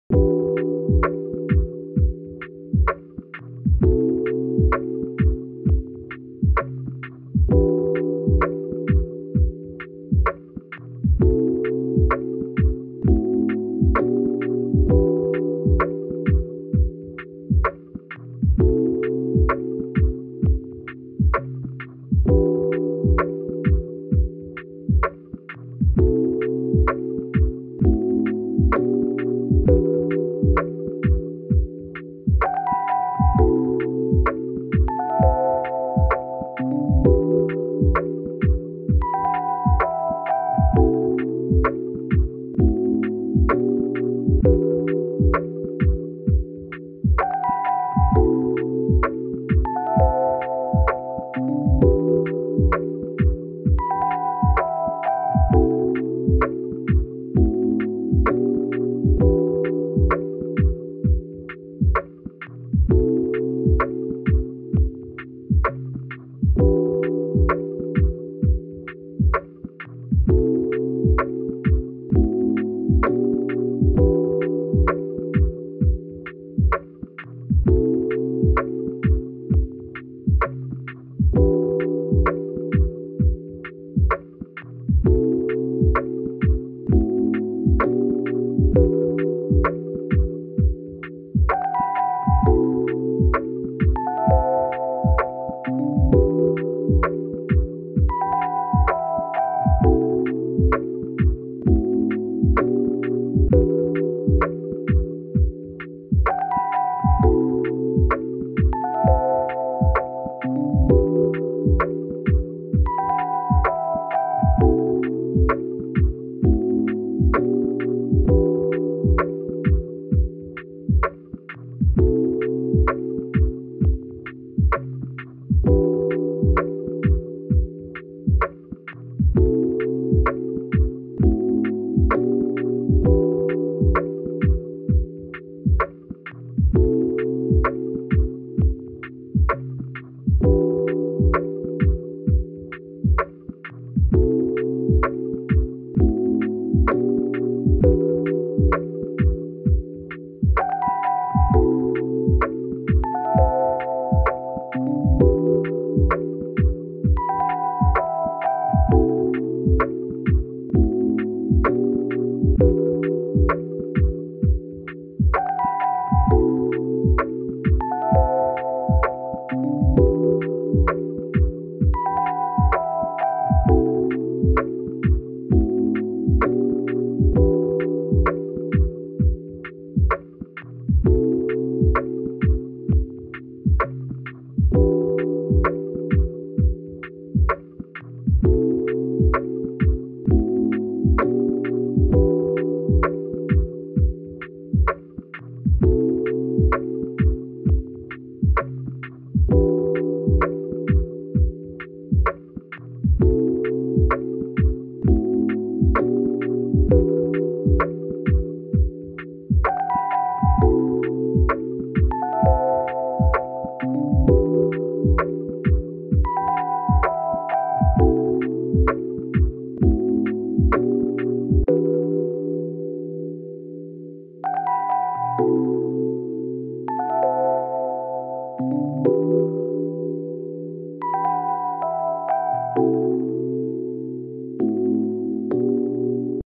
Lofi Study Beat from the Lofi CD-R Collection
A chill lofi beat to study to, straight from the Lofi CD-R Collection